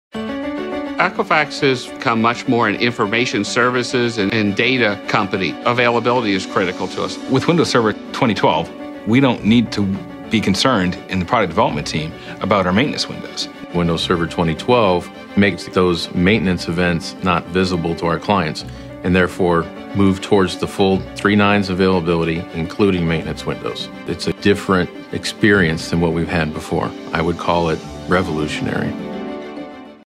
Equifax Case Study Trailer
See how Equifax is using the power of Windows Server 2012 to streamline server cluster updates, increase availability, and enhance IT innovation.